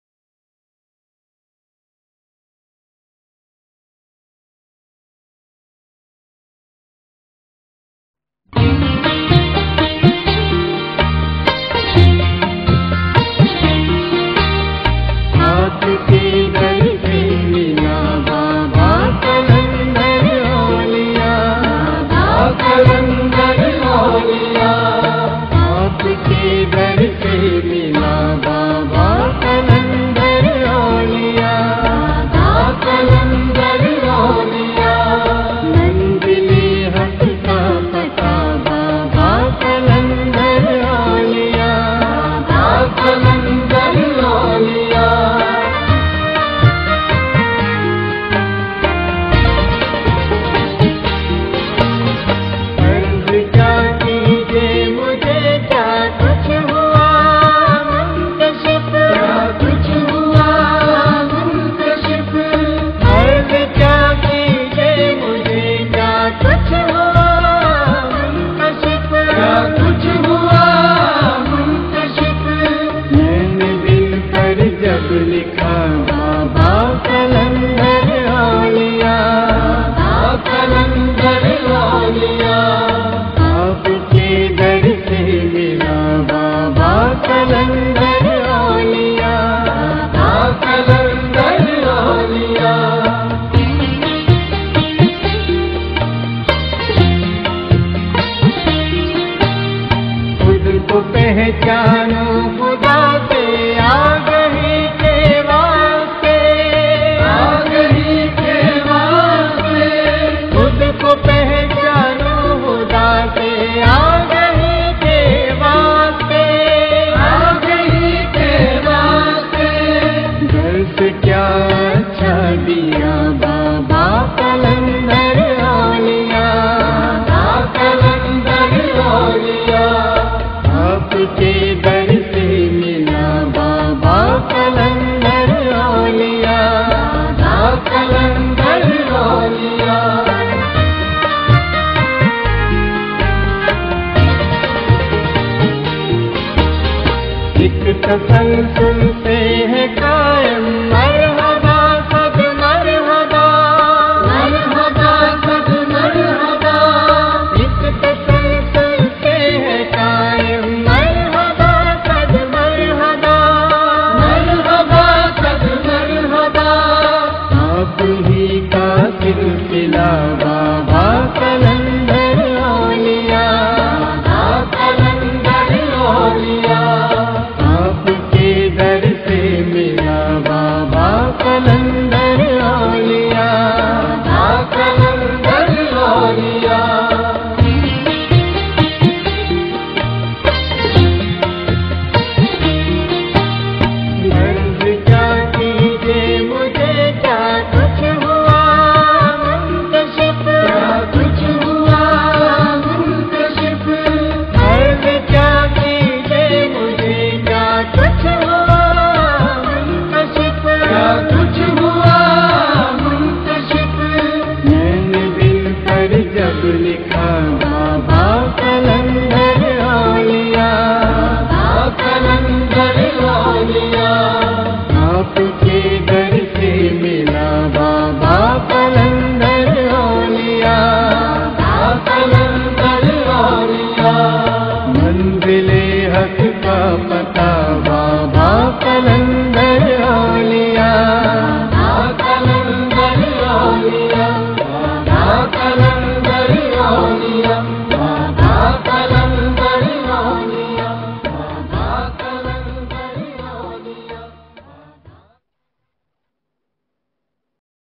Qalandar Baba Auliya Mazar Ghusl Darbar
har saal Huzoor qalanqar baba auliya k urs k program sy phely aap ki qabar mubarak ko arky gulab sy ghusl diya jata hai is saal bhi 26 janvery ko aap ki qabar mubarak ko ghusl diya gaya ghusl k bad qabary mubarak par nahe chadar charhai jati hai
is Vedio my asal qabary mubarak ki tasveer hai